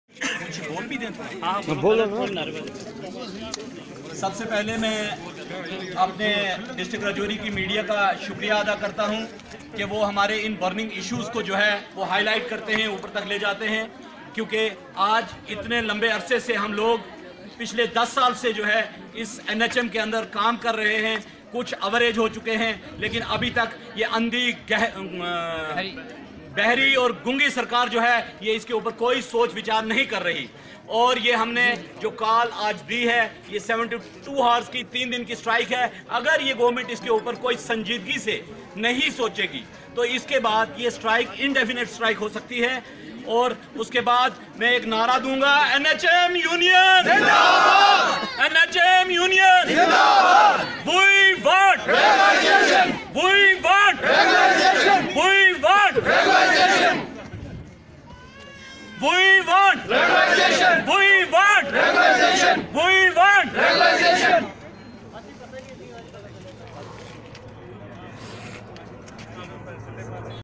NHM Employees Rajouri pen down strike & Protest in front of CMO office. AWAZ ONLINE NRWS
Video from Freelance Journalist🌹🌹🌹🌹🌹